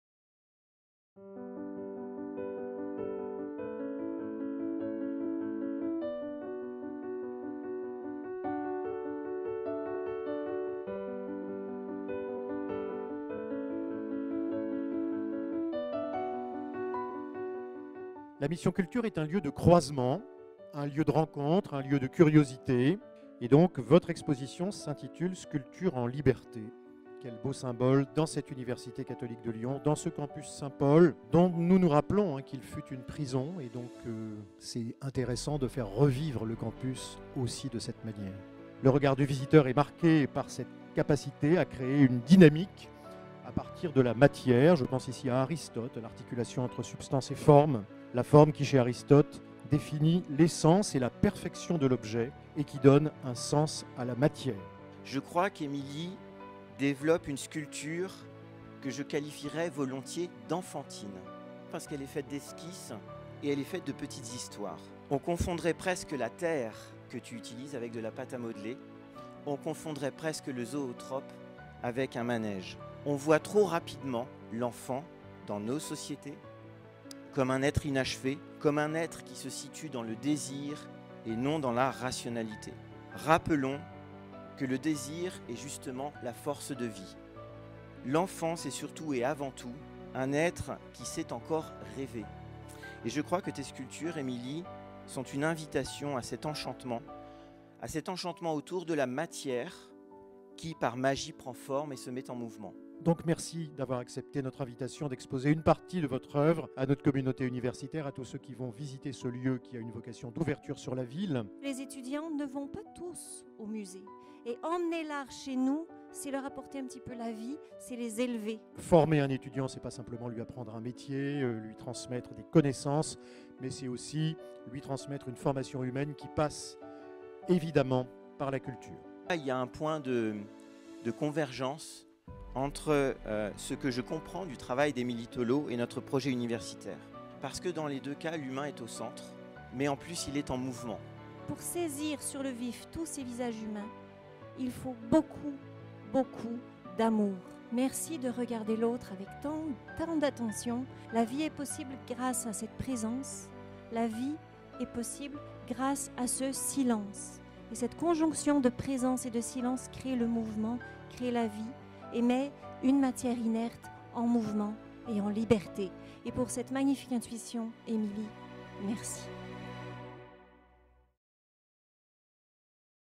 Exposition d’Émilie Tolot à l'UCLy : Sculpture en liberté
Sculptrice et réalisatrice, Émilie Tolot, a partagé son univers avec les étudiants et visiteurs du campus Saint-Paul de l'UCLy à Lyon.

Passionnée du mouvement, elle matérialise son imaginaire grâce à la Terre et compose ou décompose des corps, des silhouettes humaines.

Les personnages d’Émilie Tolot prennent alors vie sous nos yeux, nous fascinent et nous émeuvent.

Quand la sculpture rencontre la danse et donnent naissance au film animé...Une artiste aux multiples talents et à la créativité infinie que l'UCLy vous invite à découvrir du mercredi 22 mars au vendredi 12 mai 2023. Plus d'information sur l'exposition